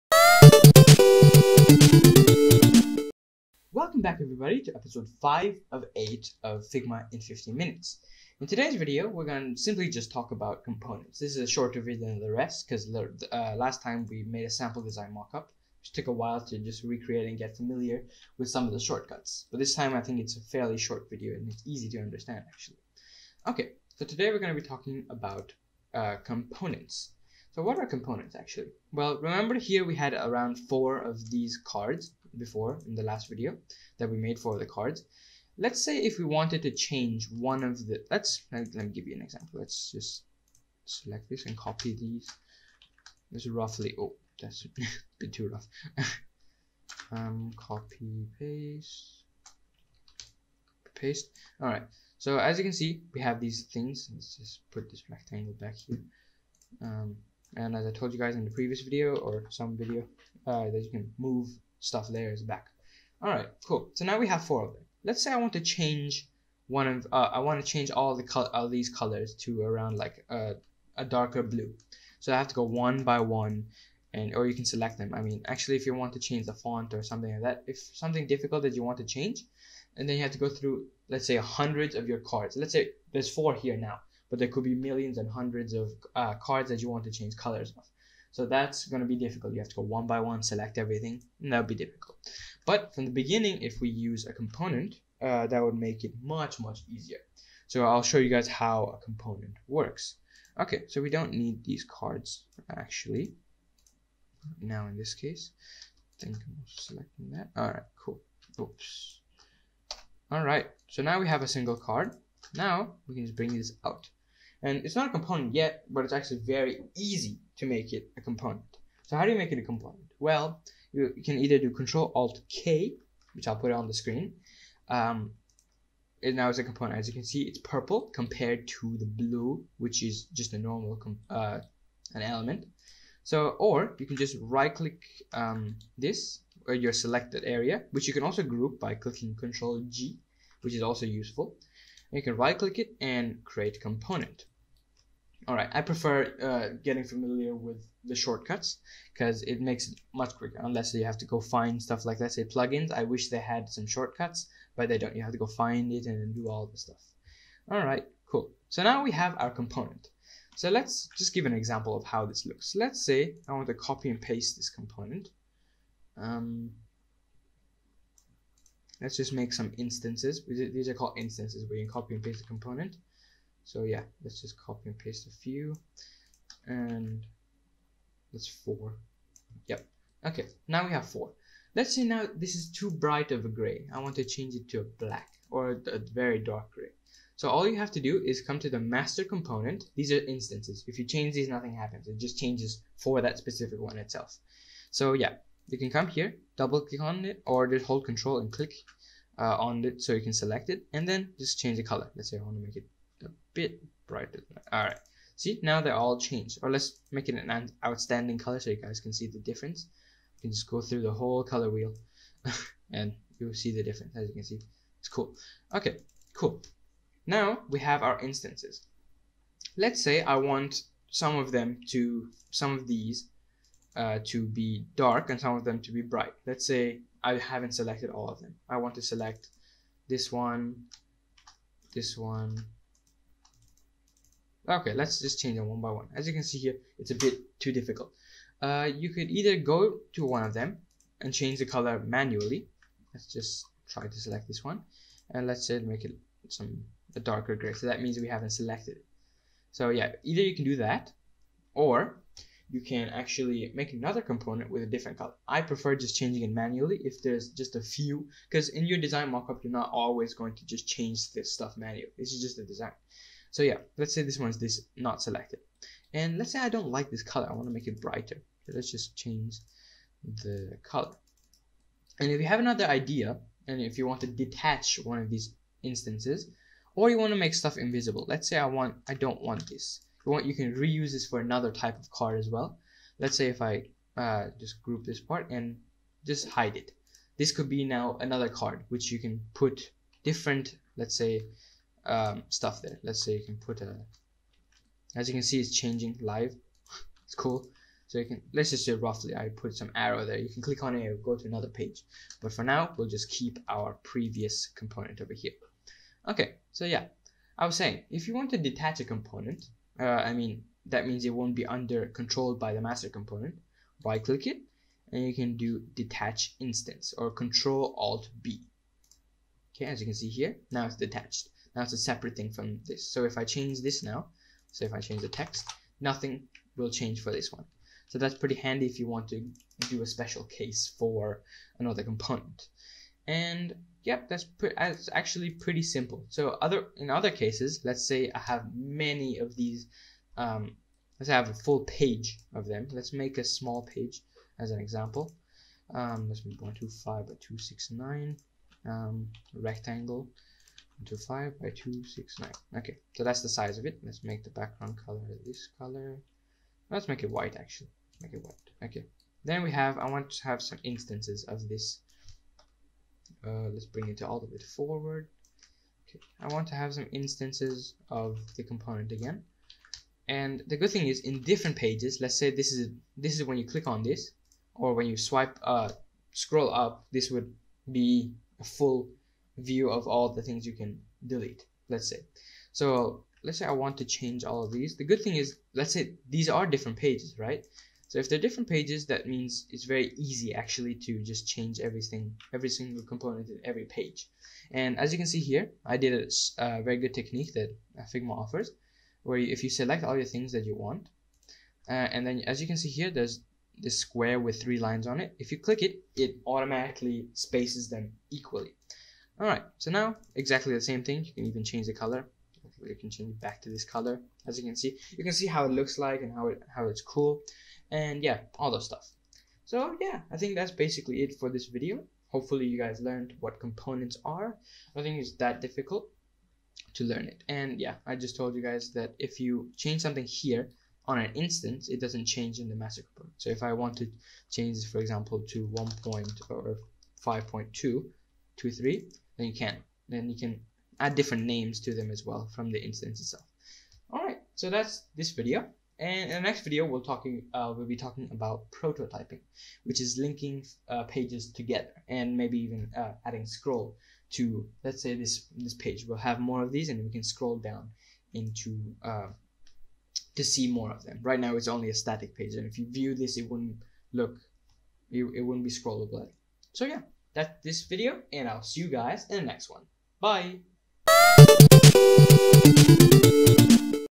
Components In FIGMA! - Figma Tutorial
Hi, my name is Arsham, I am a 15 years old developer. I have developed many things from websites to games. In these series of 8 videos, I will be going over the basics of what is Figma? Hope you guys enjoy it